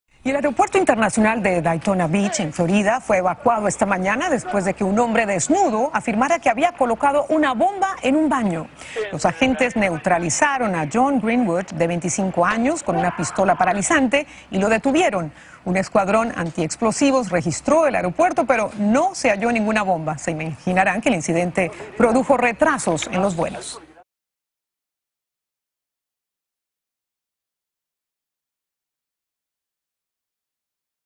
Hombre desnudo provocó pánico en aeropuerto al afirmar que había colocado una bomba en un baño
Agentes neutralizaron al hombre de 25 años con una pistola paralizante y lo detuvieron.